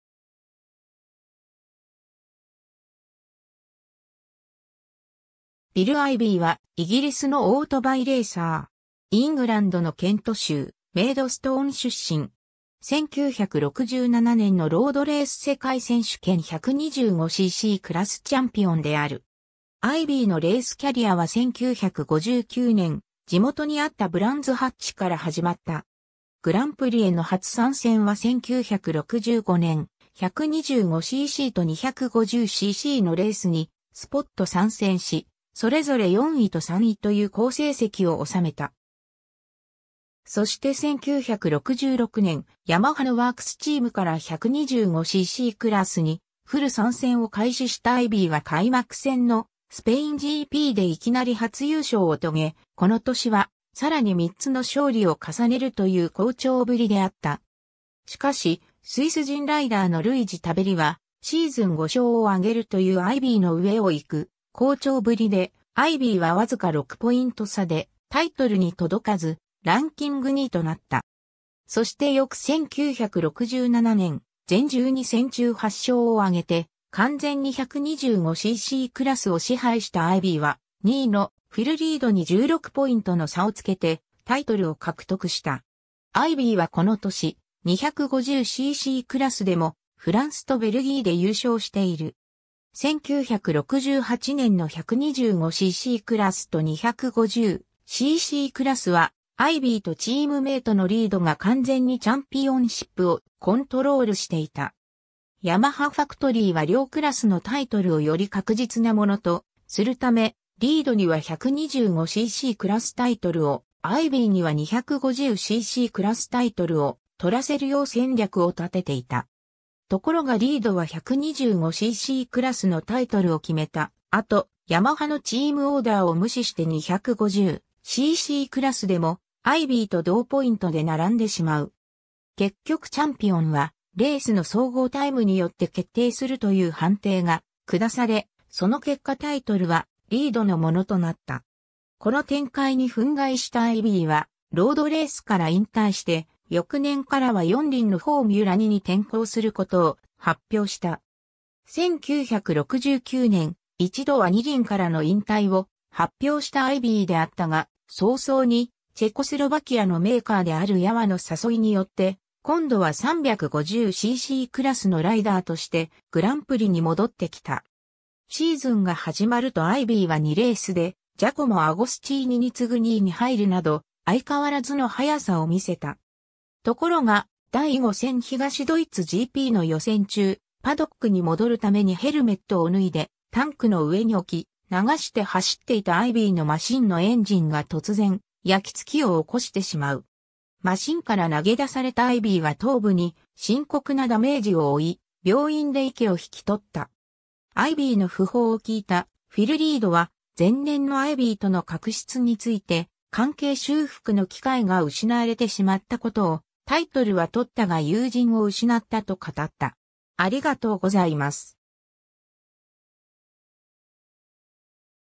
ビル・アイビー
ビル・アイビー（William "Bill" David Ivy, 1942年8月27日 - 1969年7月12日）はイギリスのオートバイレーサー。
イングランドのケント州メイドストーン出身。
1967年のロードレース世界選手権125ccクラスチャンピオンである。
 アイビーのレースキャリアは1959年、地元にあったブランズ・ハッチから始まった。
グランプリへの初参戦は1965年、125ccと250ccのレースにスポット参戦し、それぞれ4位と3位という好成績を収めた。
そして1966年、ヤマハのワークス・チームから125ccクラスにフル参戦を開始したアイビーは開幕戦のスペインGPでいきなり初優勝を遂げ、この年は更に三つの勝利を重ねるという好調ぶりであった。
しかし、スイス人ライダーのルイジ・タベリはシーズン5勝を挙げるというアイビーの上を行く好調ぶりで、アイビーはわずか6ポイント差でタイトルに届かずランキング2位となった。
 そして翌1967年、全12戦中8勝を挙げて完全に125ccクラスを支配したアイビーは、2位のフィル・リードに16ポイントの差をつけてタイトルを獲得した。
アイビーはこの年、250ccクラスでもフランスとベルギーで優勝している。
 1968年の125ccクラスと250ccクラスは、アイビーとチームメイトのリードが完全にチャンピオンシップをコントロールしていた。
ヤマハ・ファクトリーは両クラスのタイトルをより確実なものとするため、リードには125ccクラスタイトルを、アイビーには250ccクラスタイトルを獲らせるよう戦略を立てていた。
ところがリードは125ccクラスのタイトルを決めた後、ヤマハのチームオーダーを無視して250ccクラスでもアイビーと同ポイントで並んでしまう。
結局チャンピオンはレースの総合タイムによって決定するという判定が下され、その結果タイトルはリードのものとなった。
この展開に憤慨したアイビーは、ロードレースから引退して翌年からは4輪のフォーミュラ2に転向することを発表した。
 1969年、1度は2輪からの引退を発表したアイビーであったが、早々にチェコスロバキアのメーカーであるヤワの誘いによって、今度は350ccクラスのライダーとしてグランプリに戻ってきた。
シーズンが始まるとアイビーは2レースでジャコモ・アゴスチーニに次ぐ2位に入るなど、相変わらずの速さを見せた。
ところが第5戦東ドイツGPの予選中、パドックに戻るためにヘルメットを脱いでタンクの上に置き、流して走っていたアイビーのマシンのエンジンが突然焼きつきを起こしてしまう。
マシンから投げ出されたアイビーは頭部に深刻なダメージを負い、病院で息を引き取った。
 アイビーの訃報を聞いたフィル・リードは、前年のアイビーとの確執について関係修復の機会が失われてしまったことを「タイトルは獲ったが友人を失った」と語った。